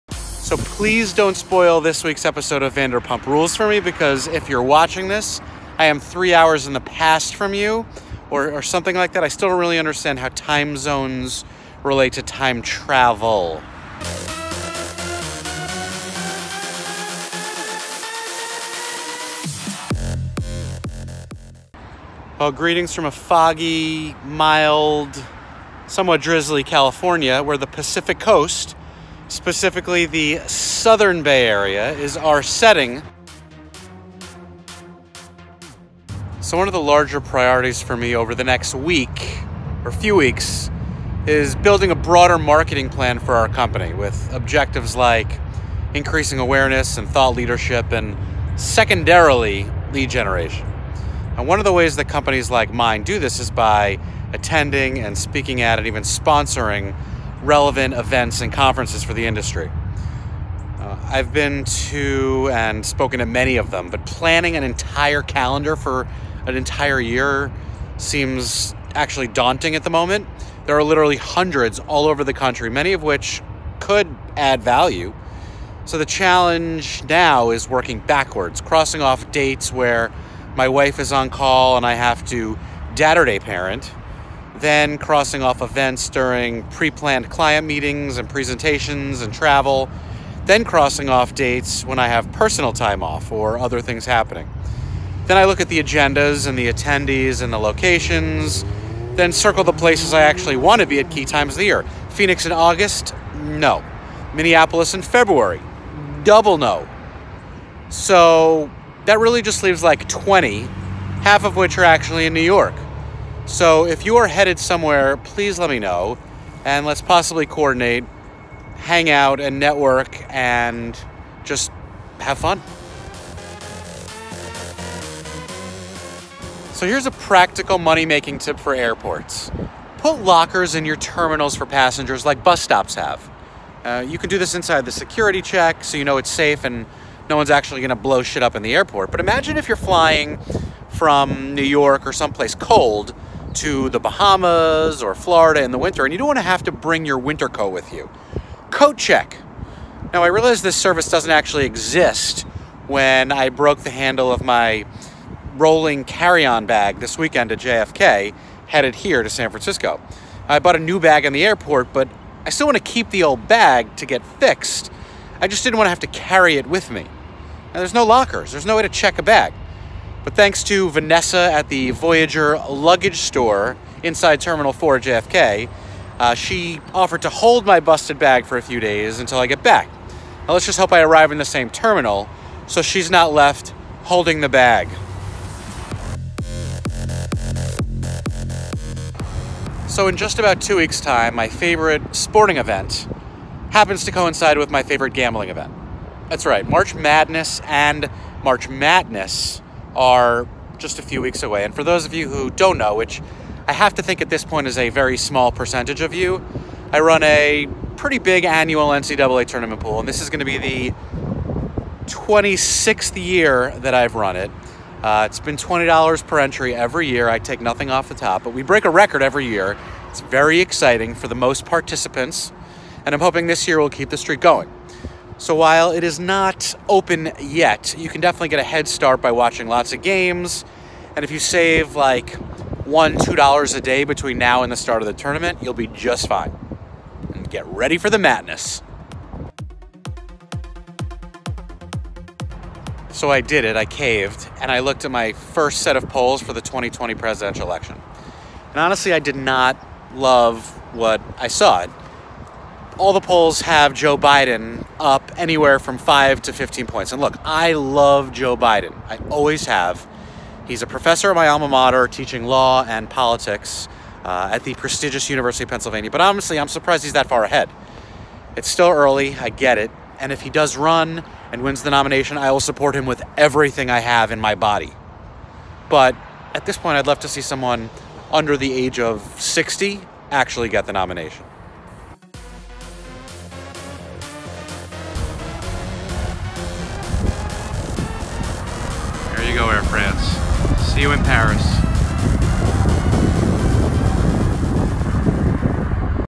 Four P's Episode 65: The PPPPacific Coast Edition
1. Planning a Conference/Events Calendar.
2. Why Aren't There Lockers in Airports?.
3. Two weeks Until the Mattness.
4. Biden... His Time.